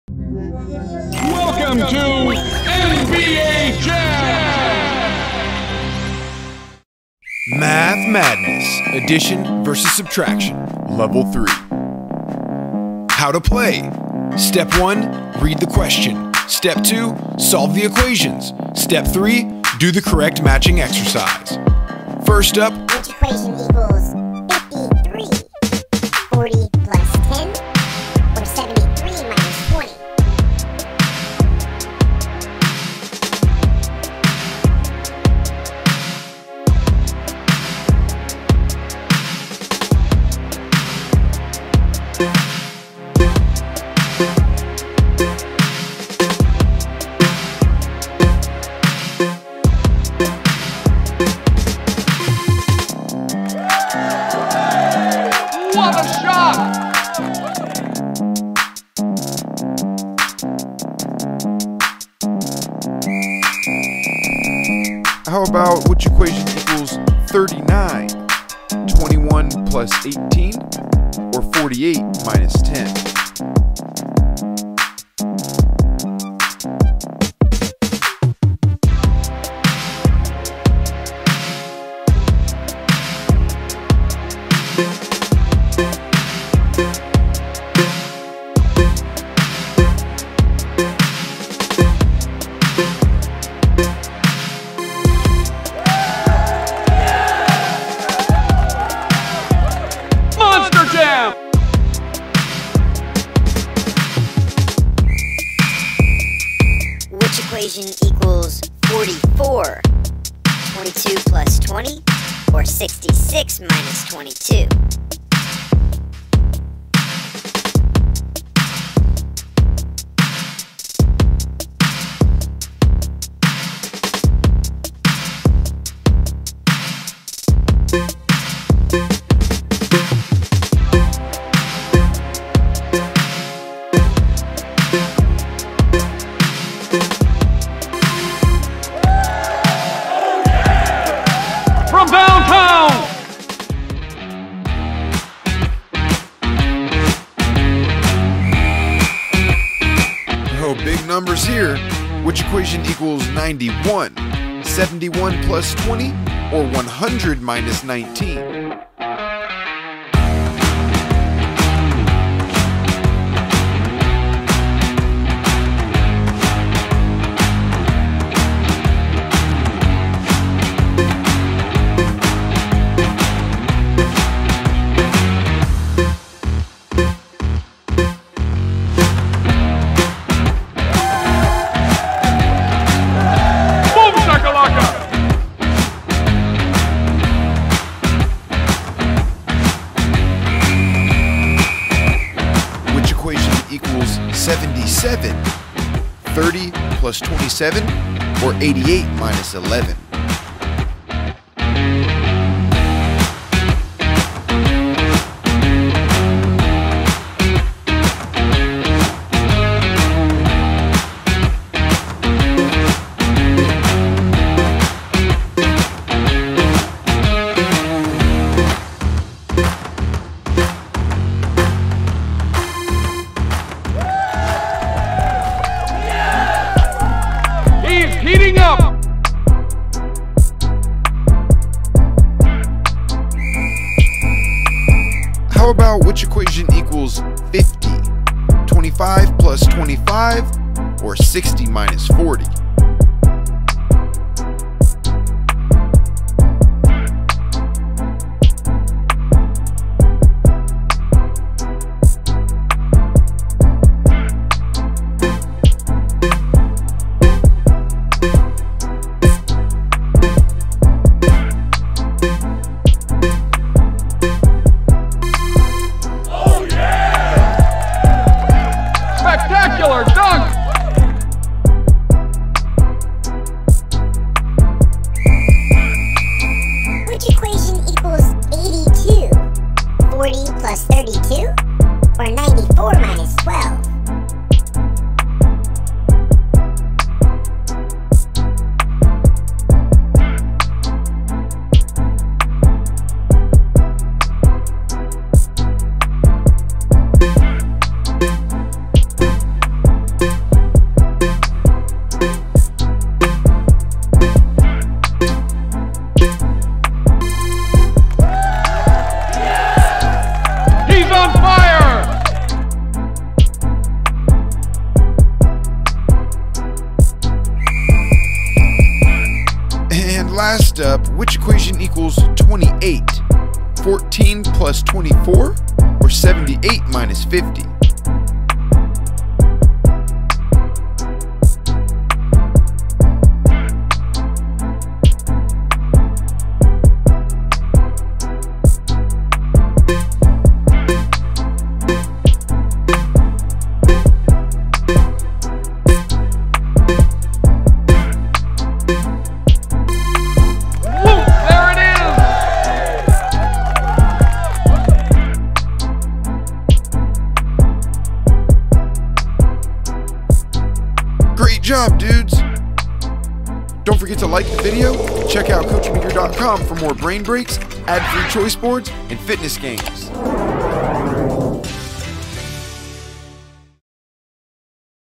This or That Fitness: Math Madness Level 3 (Addition vs. Subtraction)
This or That Fitness : March Math Madness Brain Game
(Level 3. Addition Vs. Subtraction)
2nd-5th warmup / brain break fitness game.

1.) Look at the problem.
2.) Solve the equations.
3.) Pick the this or that exercise that matches the correct answer!